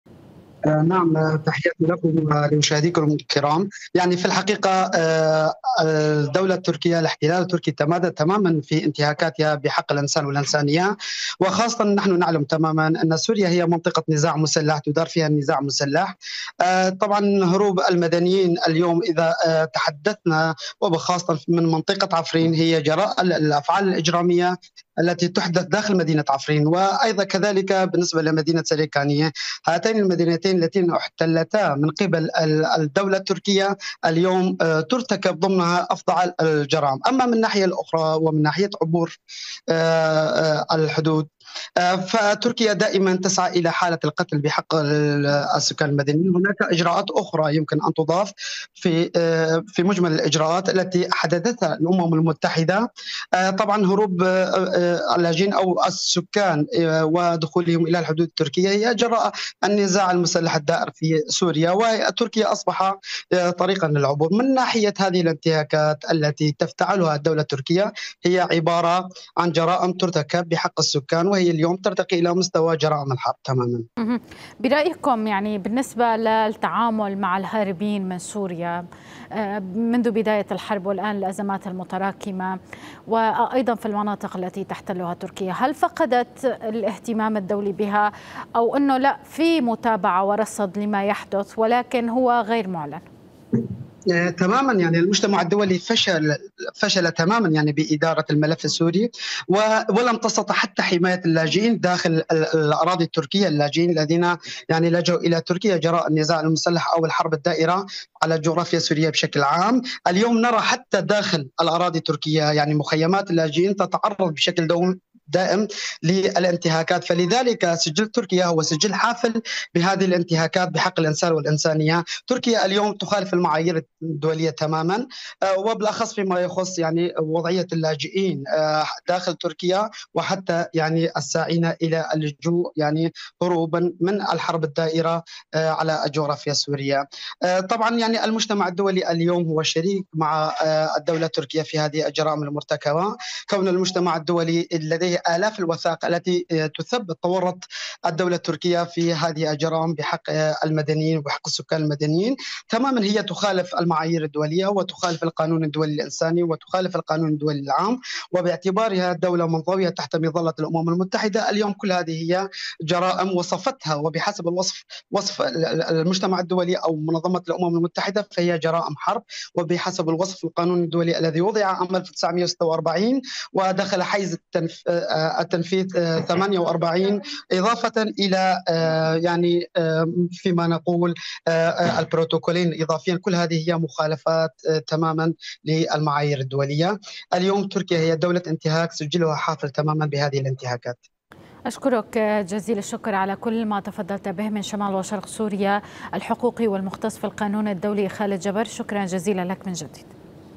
خالد جبر - استمراراً للانتهاكات... "الجندرما" التركية تعتدي بالضرب على مدنيين اثنين
شمال وشرق سوريا
استمراراً للانتهاكات... "الجندرما" التركية تعتدي بالضرب على مدنيين اثنين

المناطق السورية المحتلة
الاحتلال التركي وفصائله الإرهابية يواصلون انتهاكاتهم بحق سكان عفرين

شمال وشرق سوريا
خالد جبر - حقوقي ومختص في القانون الدولي
البث الحي / البث المباشر الأعلى مشاهدة

تابعوا قناة اليوم على التردد:
12687_27500 (H) _NILESAT